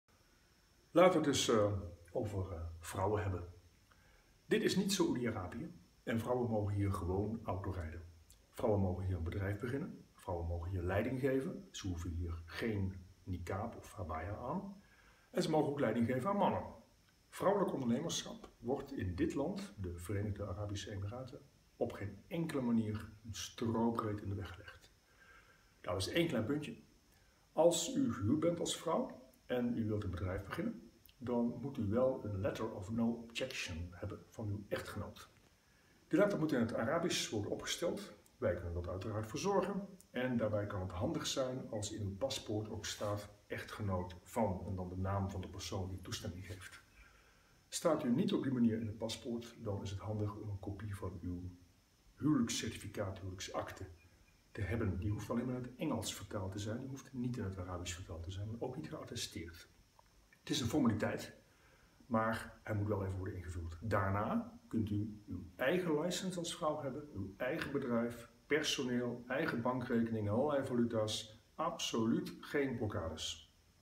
Een bedrijf starten in Dubai: vrouwelijke ondernemers
In dit filmpje vertelt specialist Paul Harts je over het ondernemersklimaat voor vrouwen in Dubai. Klein geheimpje van een vrouwelijke ondernemer: Dubai is veel vrouwvriendelijker dan Nederland.